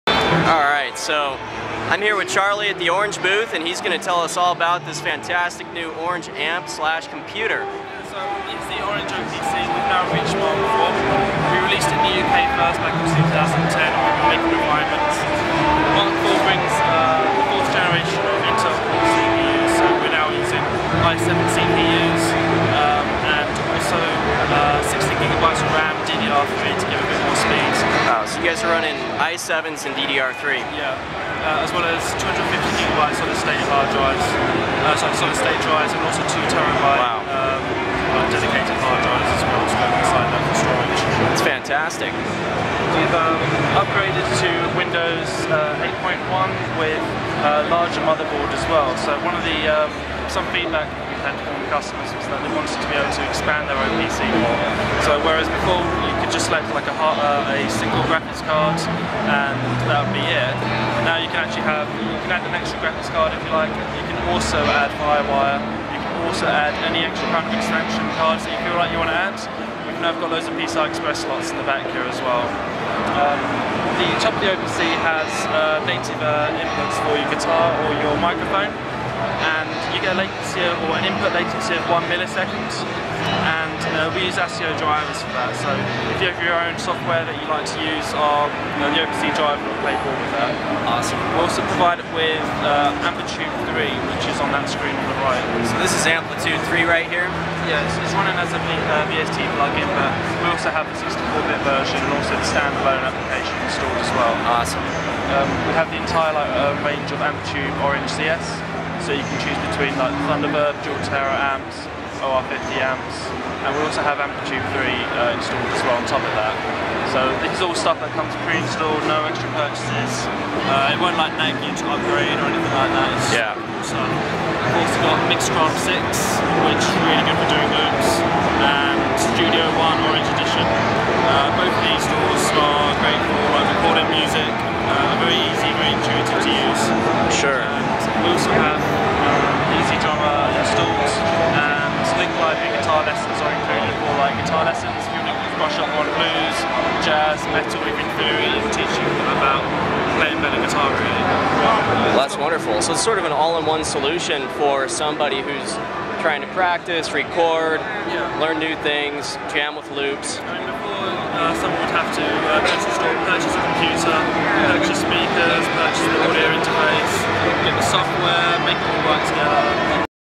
Orange OPC Computer Amplifier at Winter NAMM 2014
Charlie from Orange showed me his new OPC computer / amplifier at NAMM and it's pretty cool.  Definitely one to look up for more information.